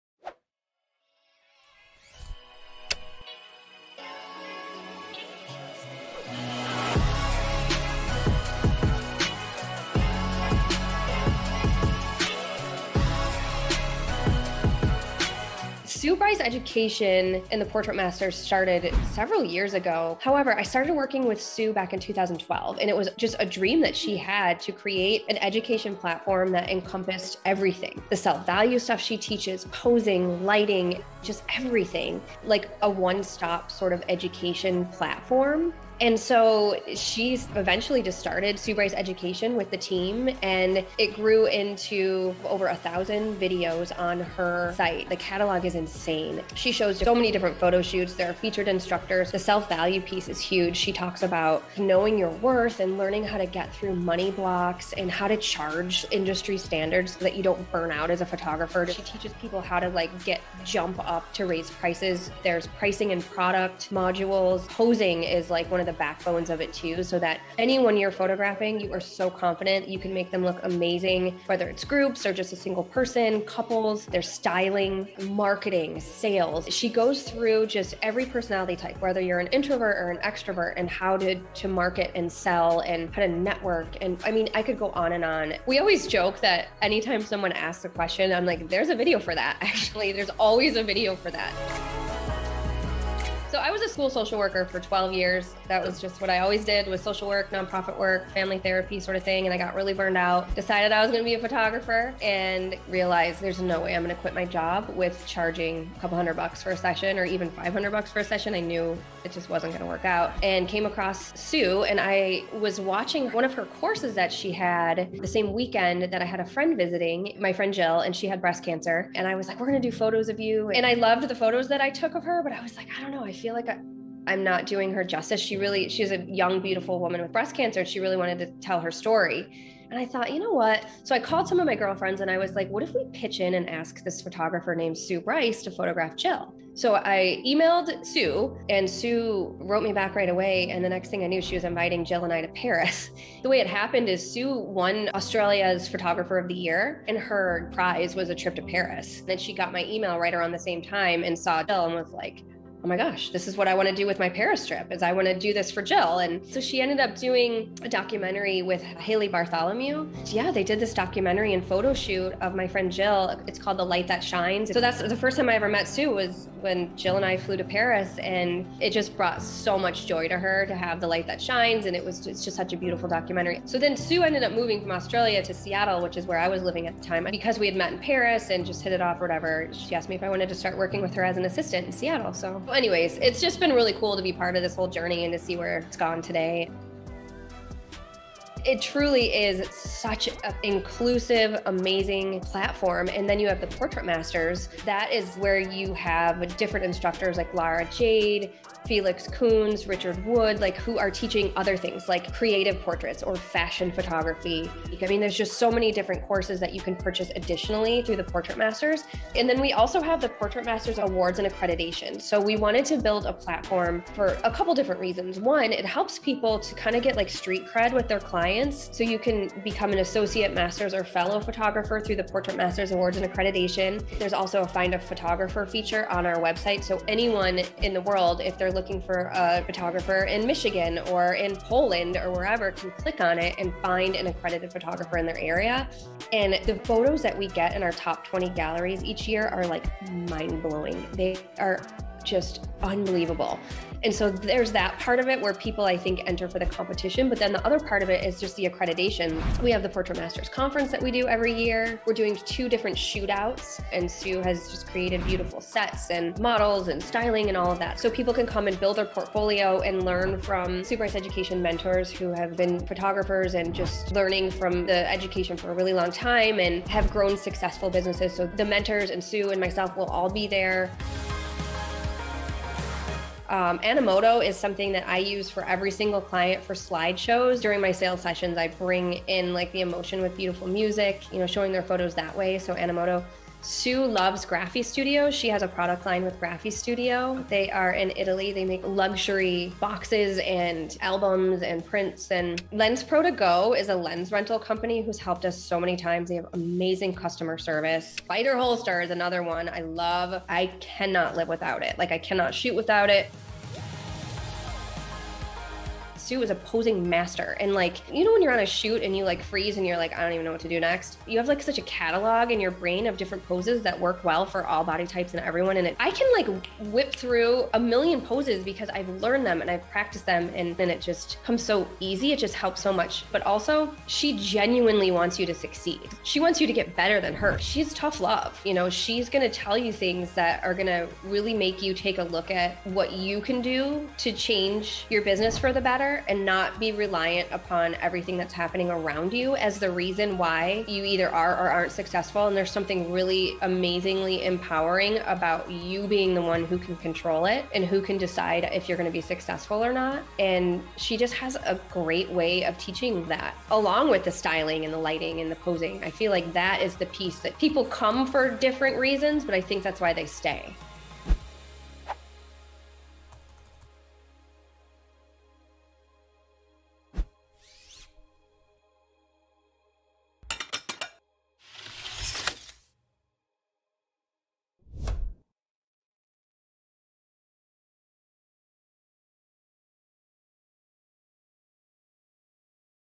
Why Photographers Need To Discover Sue Bryce Education - Vendor Spotlight With Nikki Closser
A community of cheerleaders, access to unlimited educational resources and videos taught by Sue Bryce and other photographers who are masters at their craft, and everything in between - Sue Bryce Education is a one-stop platform to help photographers grow as creatives and business owners. In our new Vendor Spotlight interview, we speak to Nikki Closser, the host of The Portrait System podcast (also powered by Sue Bryce Education). Nikki breaks down the education platform and community for us and explains why and how it’s an investment that's worth your time and money.

Nikki, who is also a photographer, shared the favorite brands and vendors that she and Sue also turn to to make their lives and workflows easier. Brands such as Animoto for client slideshows that add emotion to sales sessions with beautiful music, Graphistudio for their handmade luxury boxes, albums, and prints, LensProToGo for on-demand lens rentals and amazing customer service, Tenba bags for carrying allll the gear (Sue has designed the most stylish camera bags for them), and Spider Holster  - the camera strap neither can live (or shoot) without.